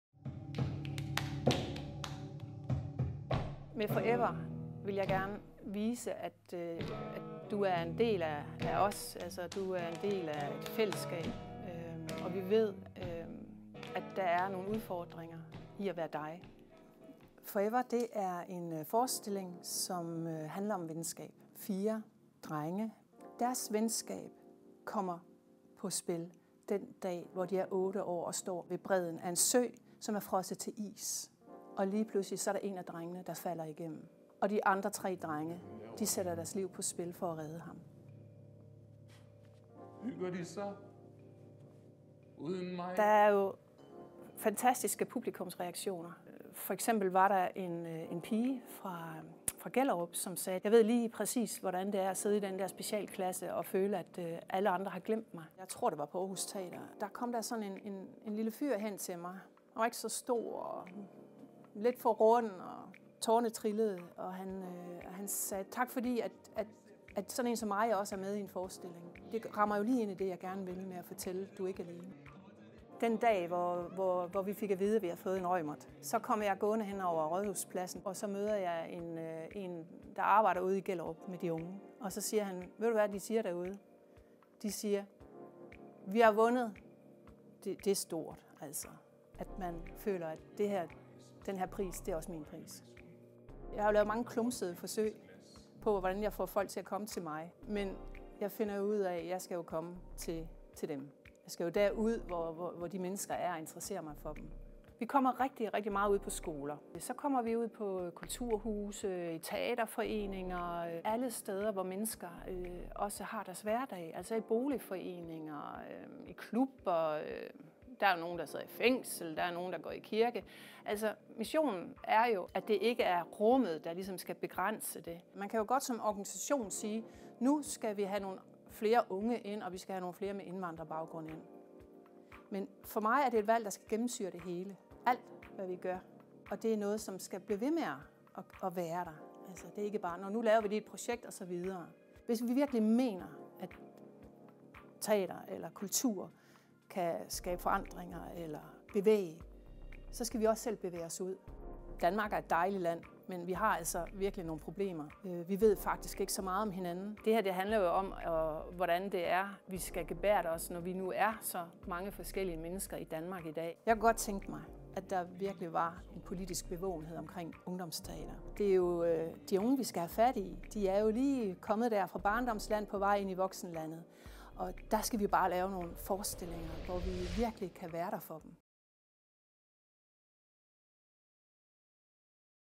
4EVER af Opgang 2
Kulturmødet Mors 2018. Scenekunsten bevæger os var en debat arrangeret af den danske teaterbranche på Limfjordsteatret.